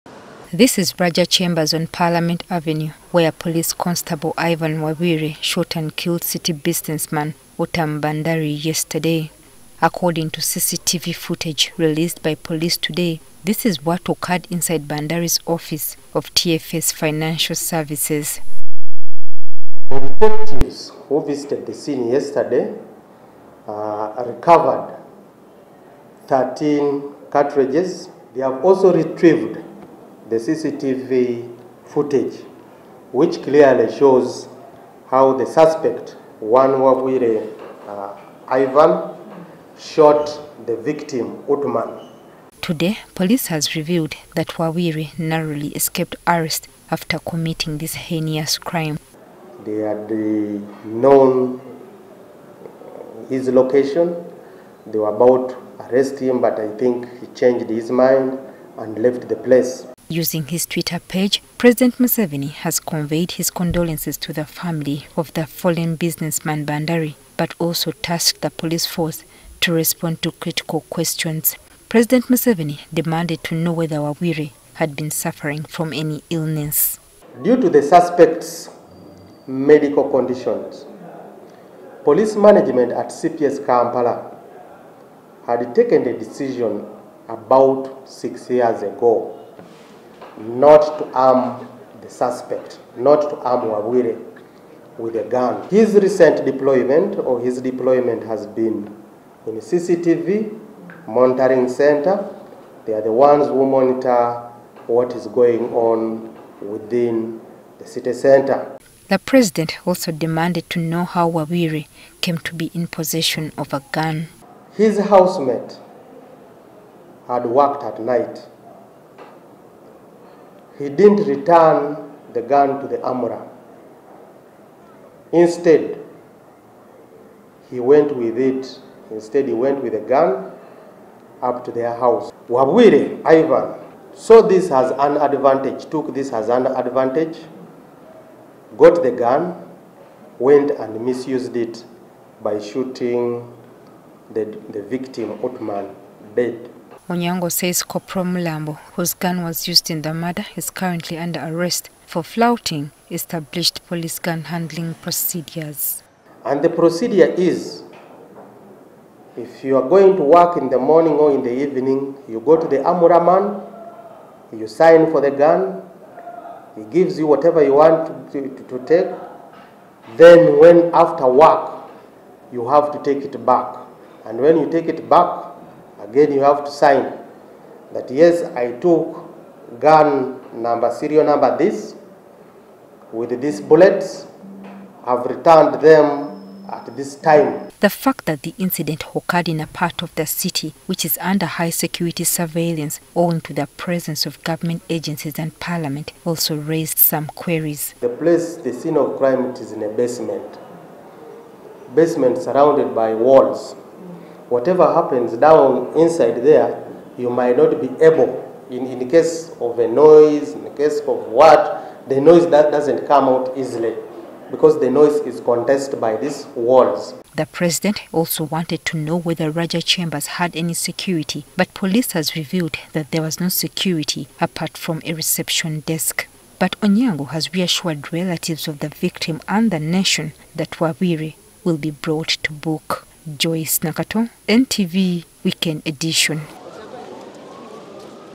Killer police officer stole gun, was unwell - Police
Police have revealed that the gun that police constable Ivan Wabwire used to shoot city businessman Uttam Bhanderi had been stolen from a colleague. According to Patrick Onyango the Kampala metropolitan police spokesperson, Wabwire stole the gun from his colleague Corporal Steven Mulambo, who had returned home with it, instead of keeping it at the police station. Mulambo has now been arrested for flaunting police gun-handling procedures while the force continues its hunt for Wabwire, who is still on the run.